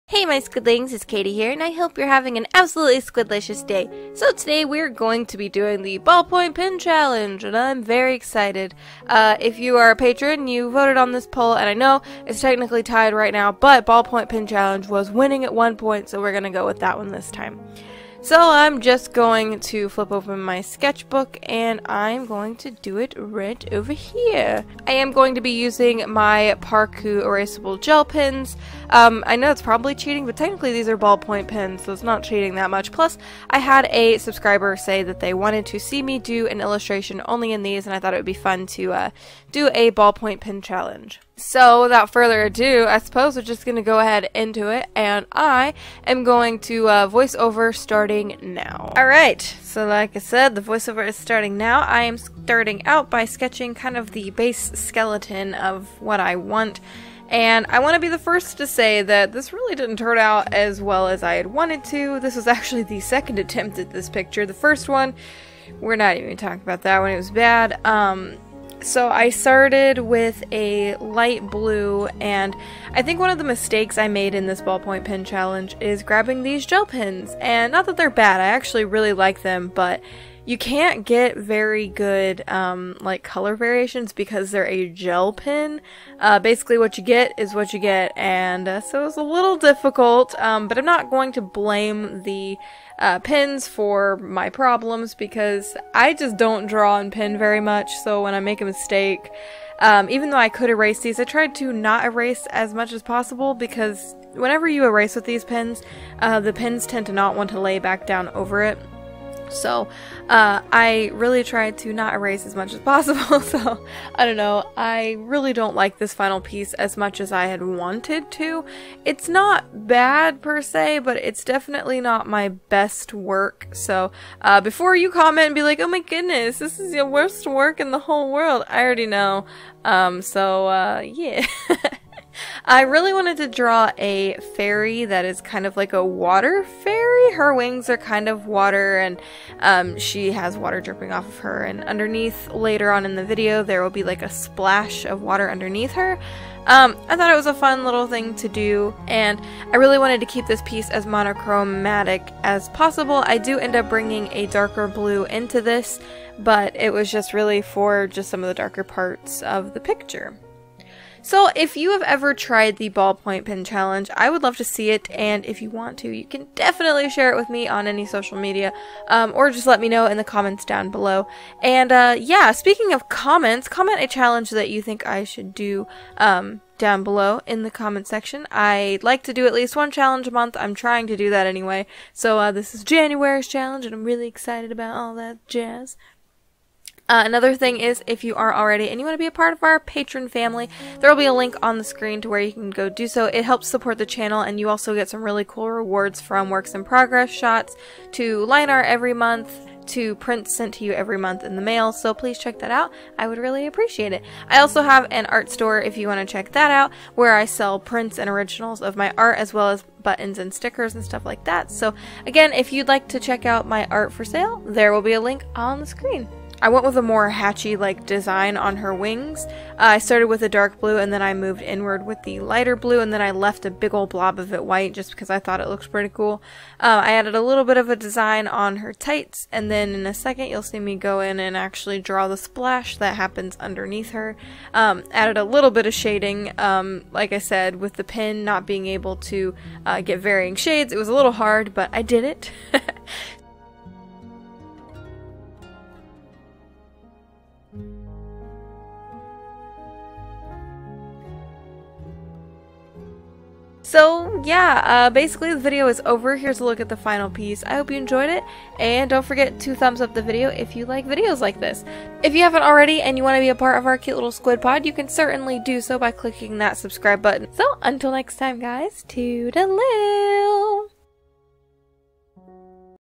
BALLPOINT PEN CHALLENGE!
Hey my Squidlings! For this month's art challenge, I thought it would be fun to do another ballpoint pen challenge!

Art materials used in this video:
My sketcbook
ParKoo Erasable Gel Pens

Amount of time taken to complete this piece:
35 minutes